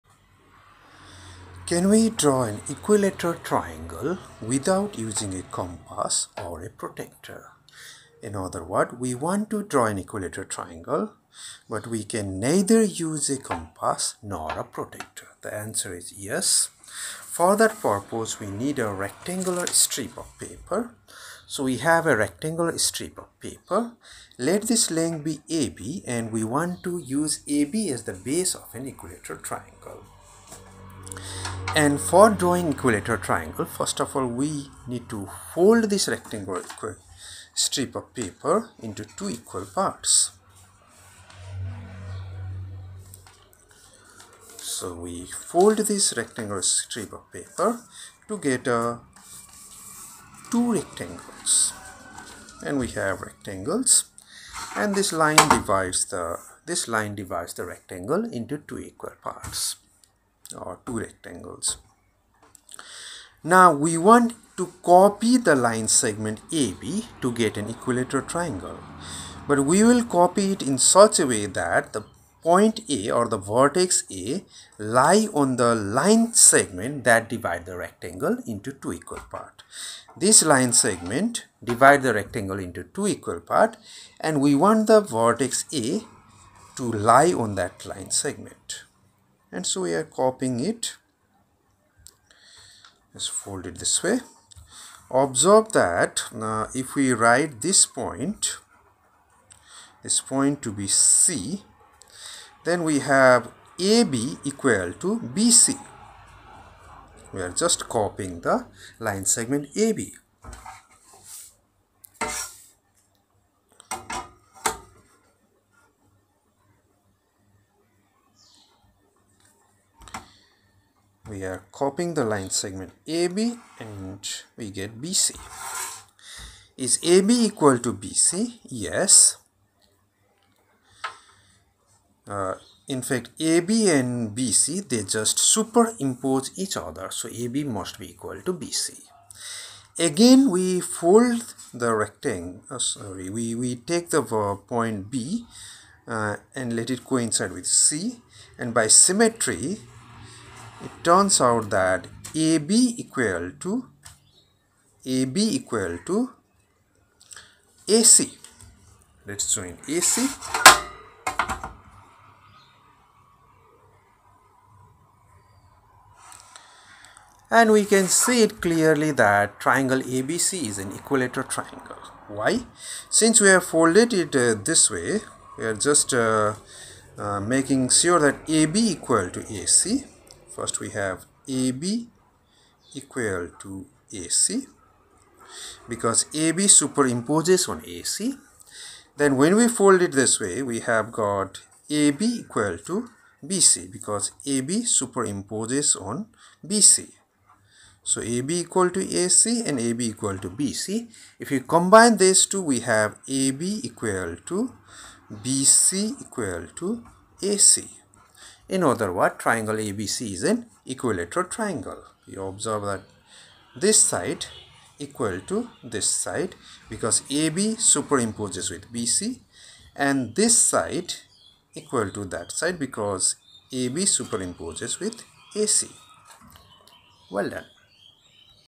Draw an equilateral triangle, without using compass and protractor
We draw an equilateral triangle, using just a rectangular  strip of  paper, a pen   and a scale only,  in the  process, we  neither  used a  compass nor a protractor.